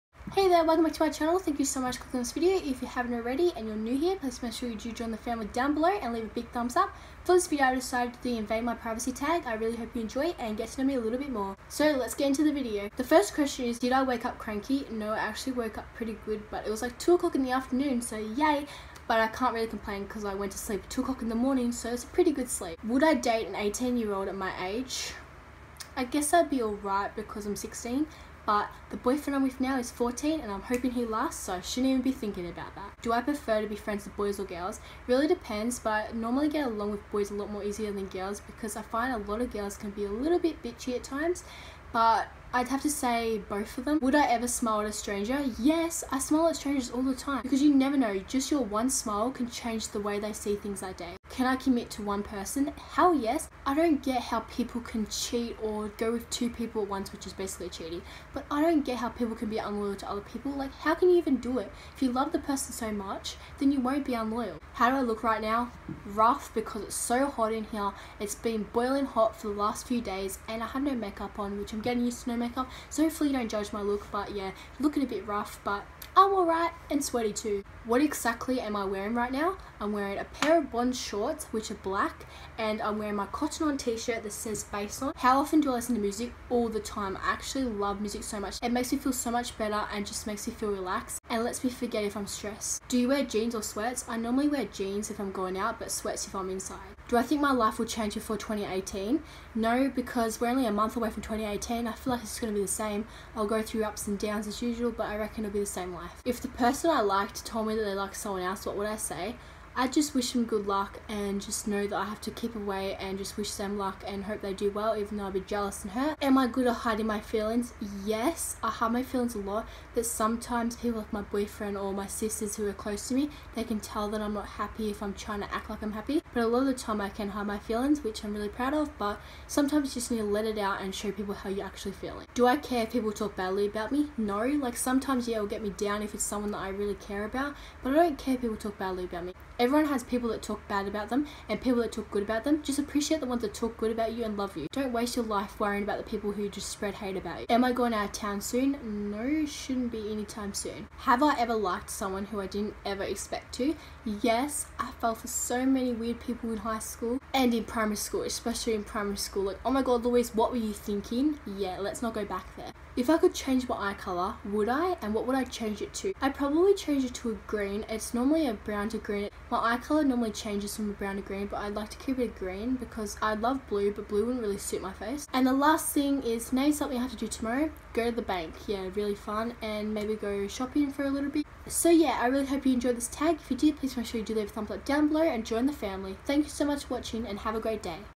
Invade my privacy tag
About me-
How old am I? I'm 17 years old
Who am I? I'm an Australian teenager who loves filming videos, playing sports and singing/dancing but sucks at the last two things I just said 😂
What do i film on? I film on my iPhone 6S
What do I use to edit? I use iMovie on my iPhone.

[Sub Count:3424]